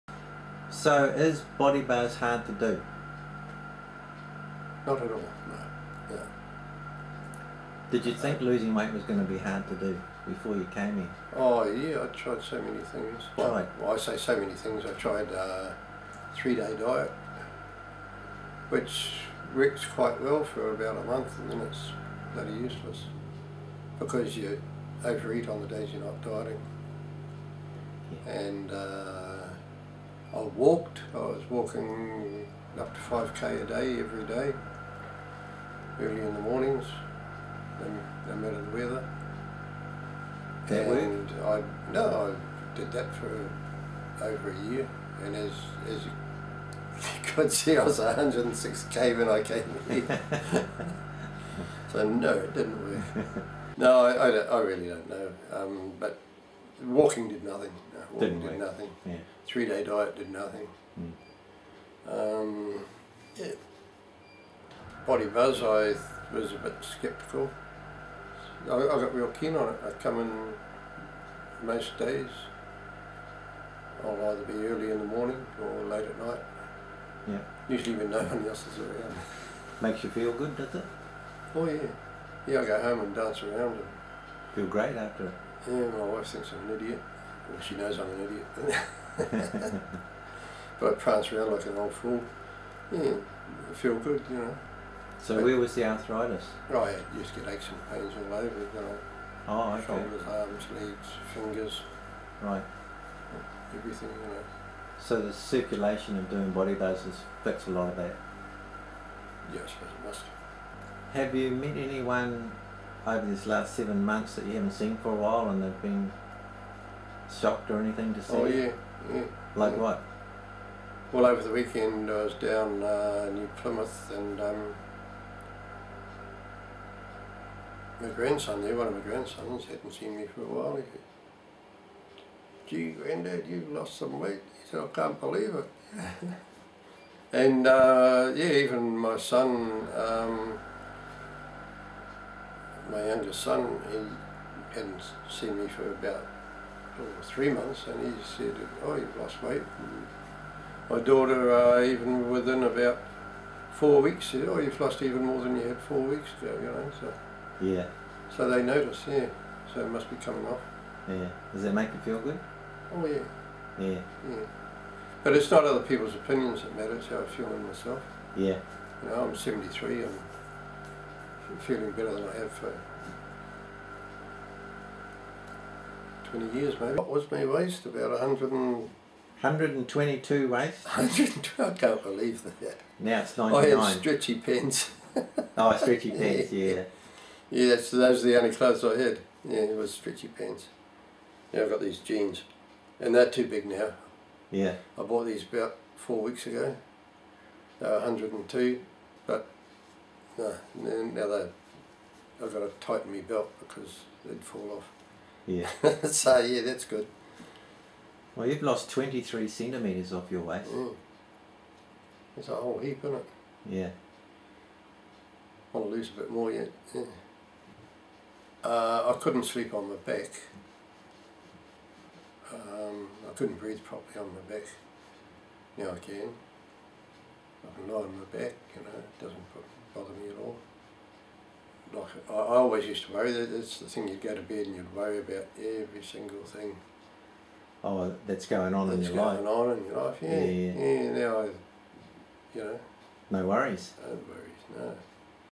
Colin lost 23 kg
Colin lost 23 kg in 7 months and shrunk his waist 23 cm.